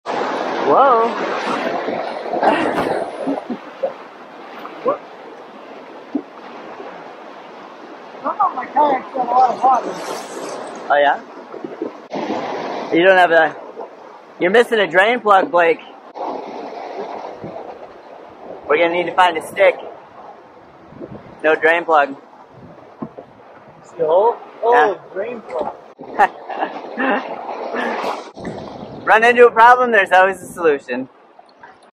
Well That's a Problem, No Drain Plug
Blake take his boat on whitewater for the first time, but forgets something important.